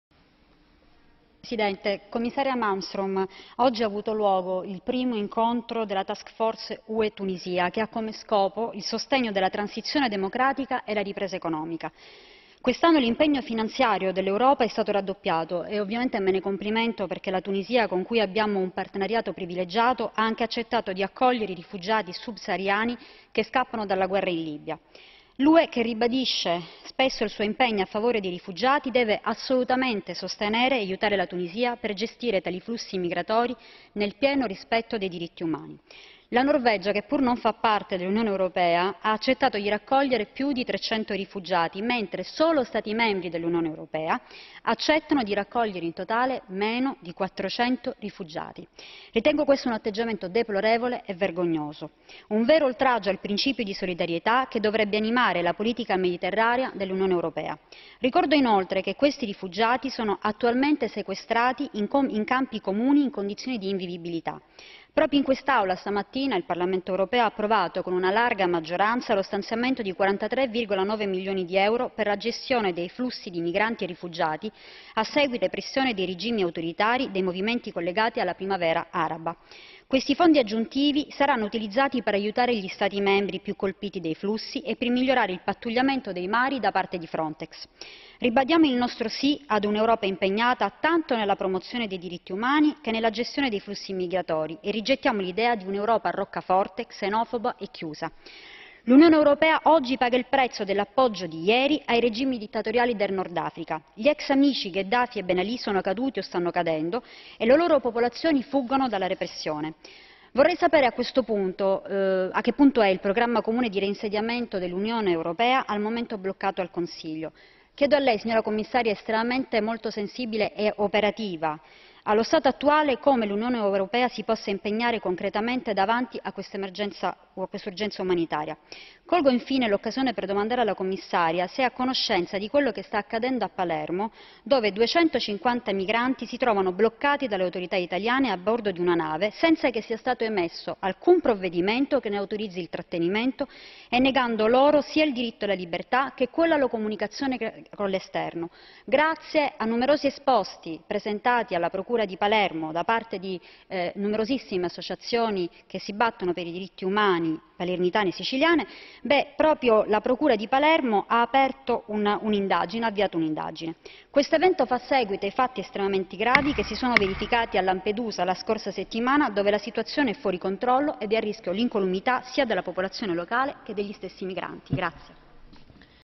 Sonia Alfano: sull'emergenza immigrazione: l'Ue aiuti la Tunisia ed i rifugiati
Sonia Alfano - Strasburgo - 28/09/11 "Emergenza immigrazione: l'Ue aiuti la Tunisia ed i rifugiati"